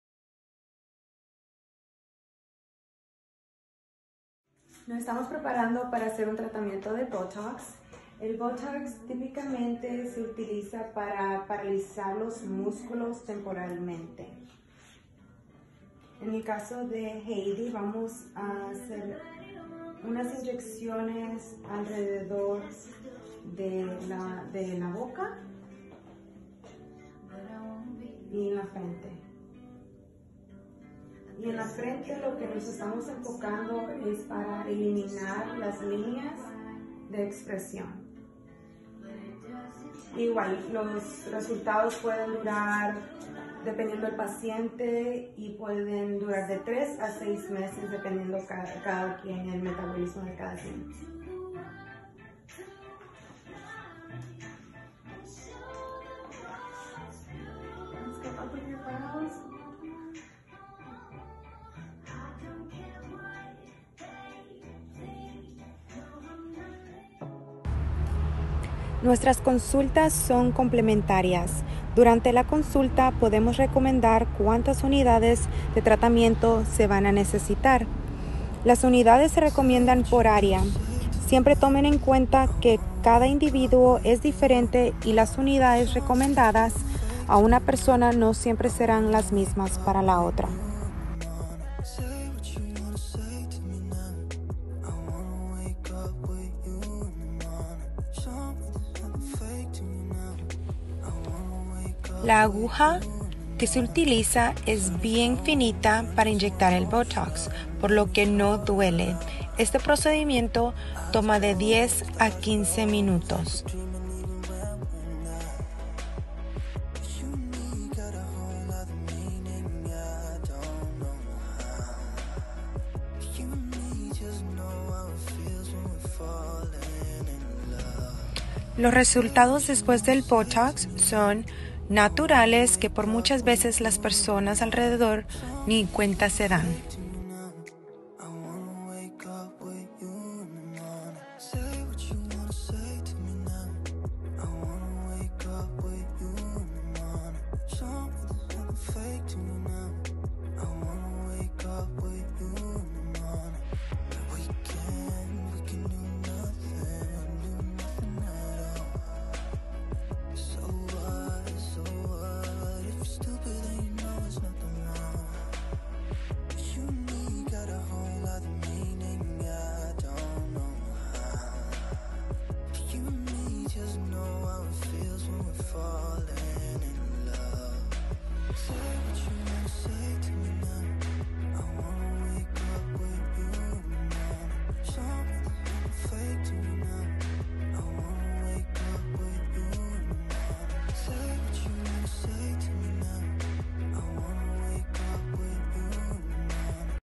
Botox for the gummy smile (Spanish Version)
Do you have a smile that shows more of your gum than you would like? There is a way to help reduce this concern. Neuromodulators like Botox or Dysport can be used to help relax the muscle slightly and help your smile.